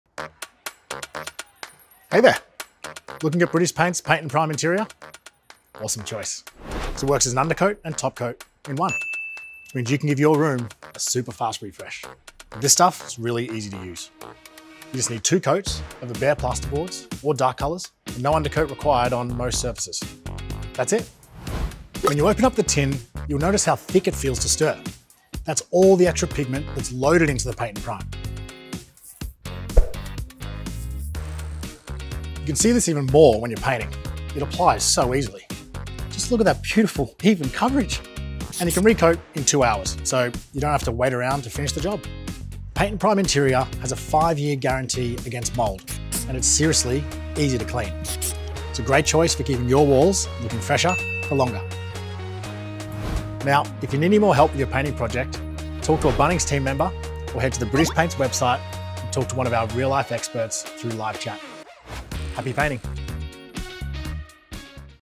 British Paints - Paint & Prime Interior
British Paints Paint & Prime Interior helps save time with an undercoat & topcoat in one. With only two, easy to apply coats, over bare plasterboard, plaster repairs, and dark colours, achieve a beautiful, easy to clean, finish for your home with this All-in-One™ paint.